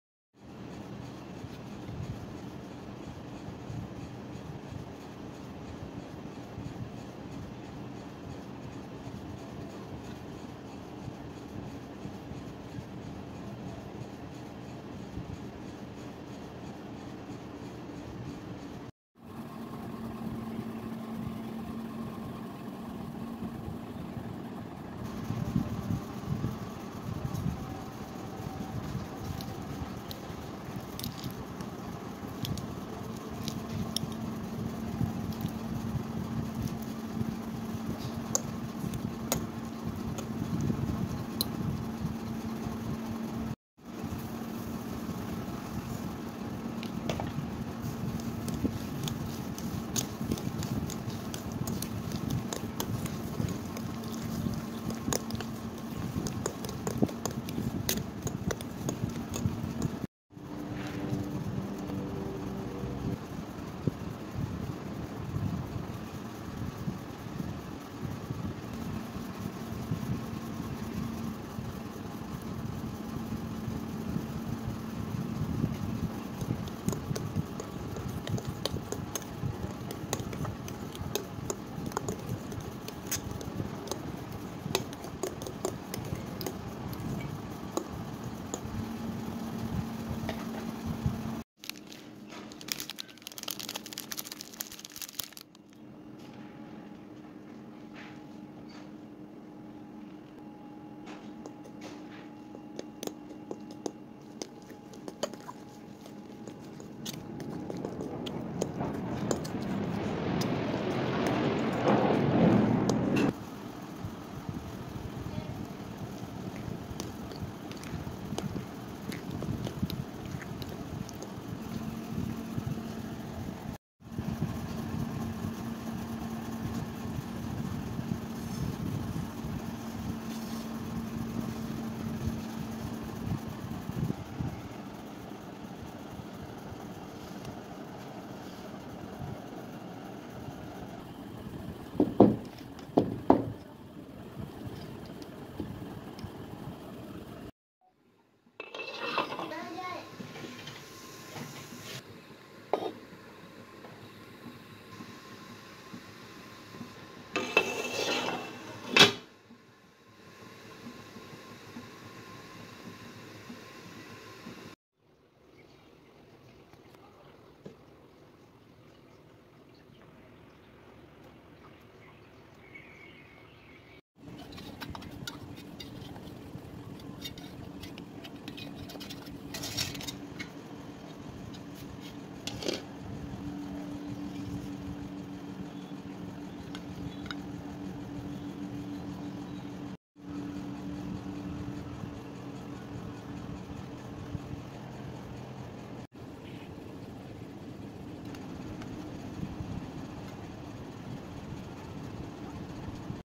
🎂ডিম ছাড়া,কলার কেক,🎂সহজে চুলায় তৈরি।টিফিন, নাস্তায় জটপট রেসিপি।Kolar cake recipe banana cake recipe.
কলার কেক সহজে তৈরি চুলায় ডিম ছাড়া। টিফিন, নাস্তা সহজ রেসিপি।